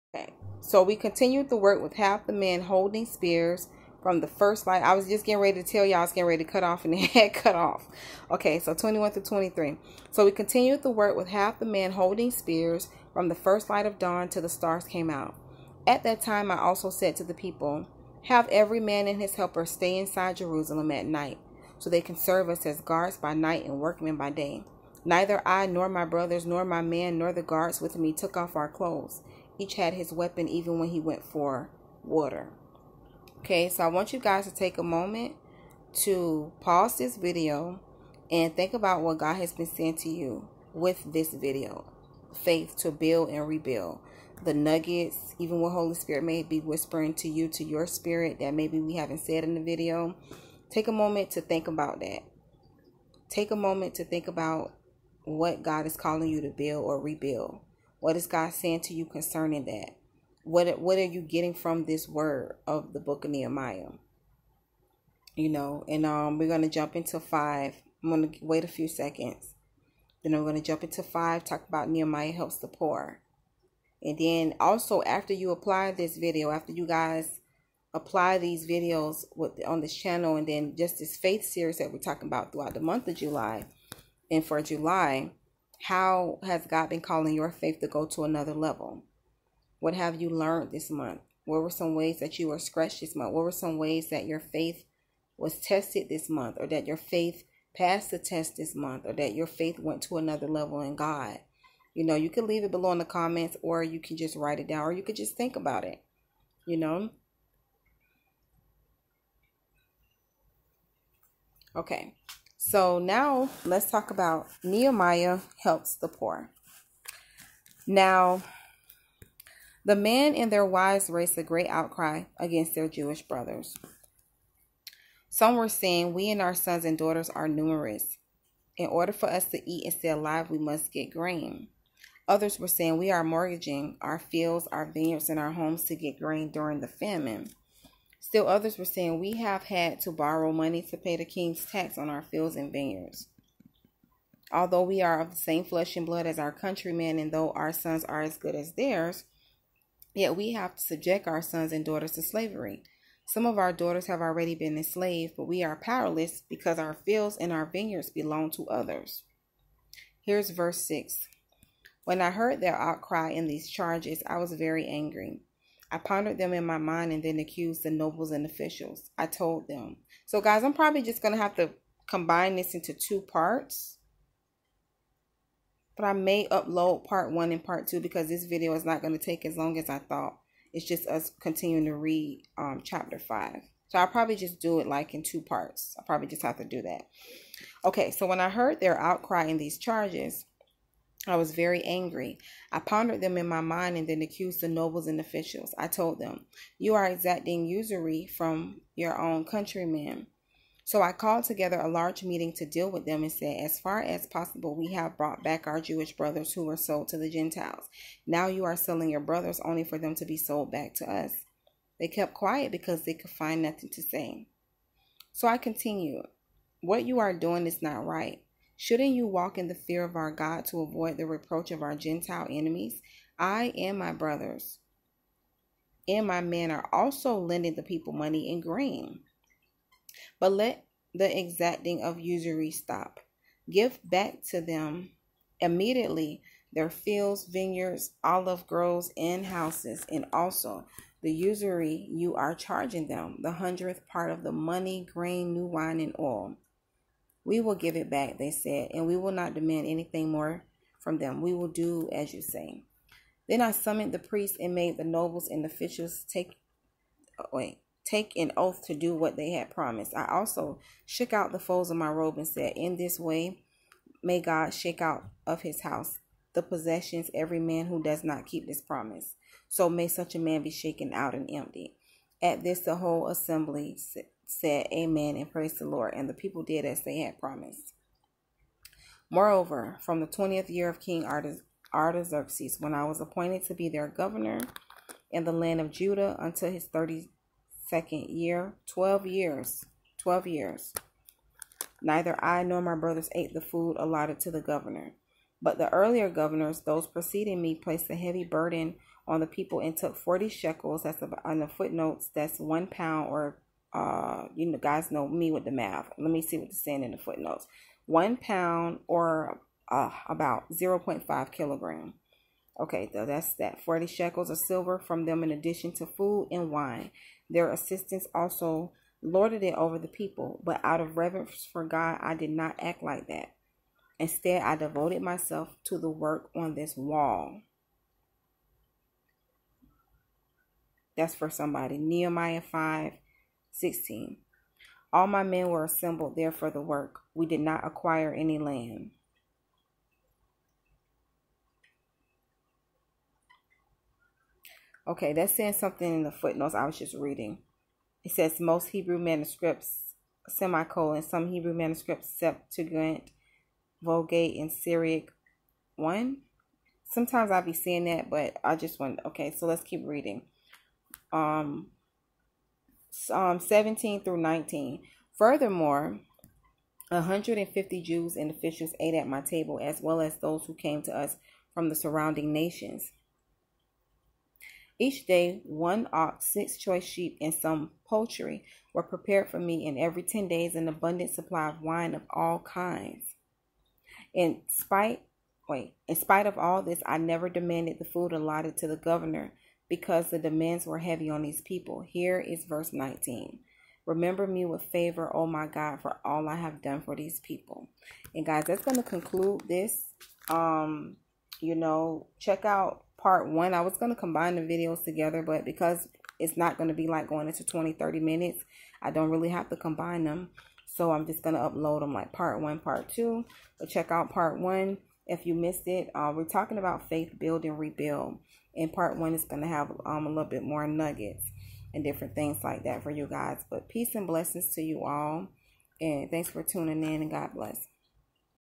🔧 FAITH: BUILD & REBUILD- PART 2|7/22/20, Wednesday
Please see Part 1 if you missed it 🙂 All Glory to God

*Scriptures: Nehemiah 4 and 5

🔧 Warring Warrior's Prayer Line:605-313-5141/ Code: 252851#

- Mentorship with Me
- The Girls Talk 2020